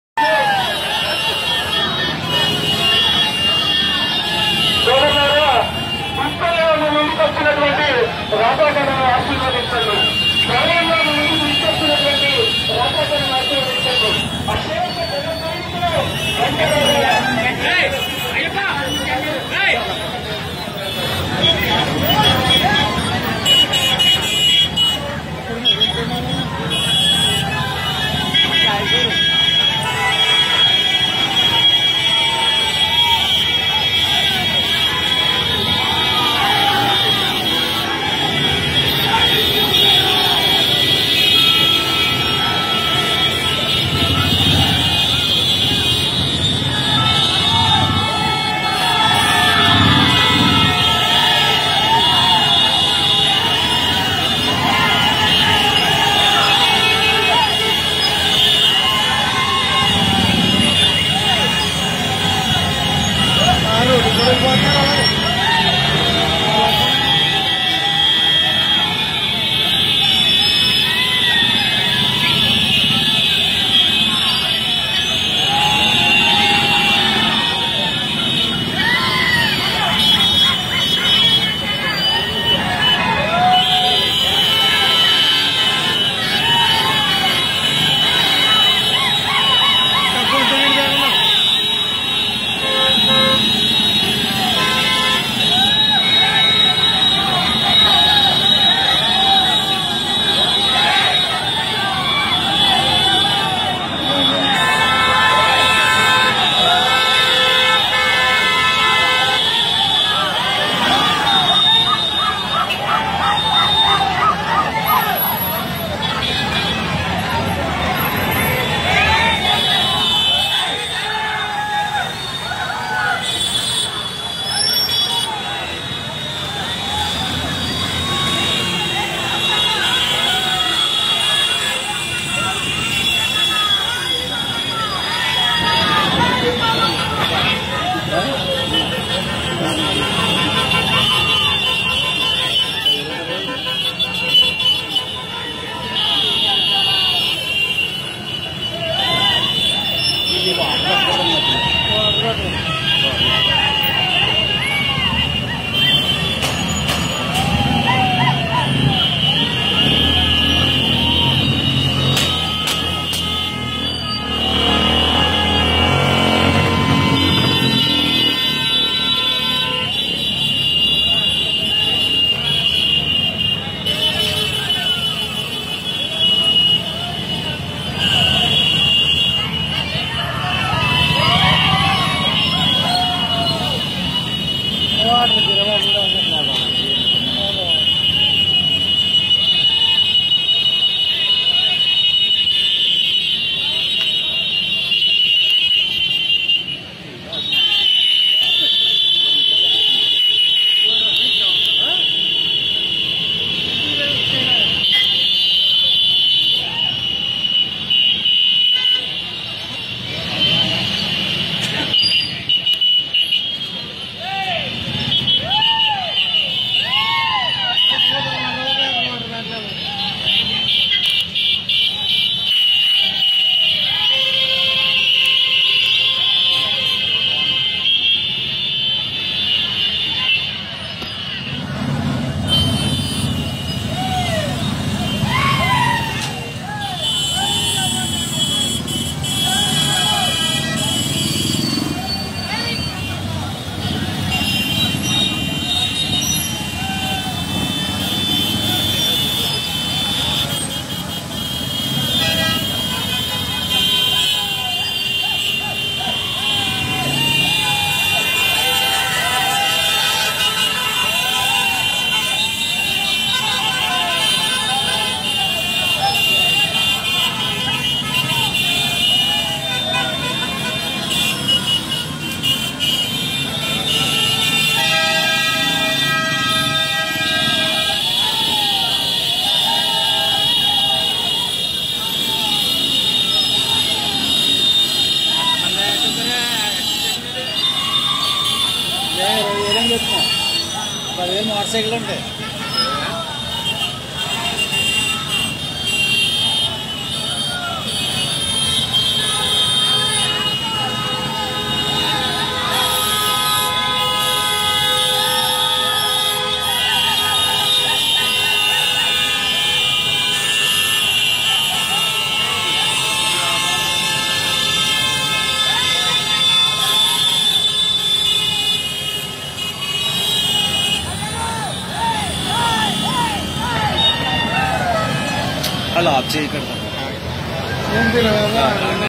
Janasena Rally at Razole
Huge crowd with bikes and janasena flags at razole constituency under the leadership of Rapaka varaprasad (MLA Candidate)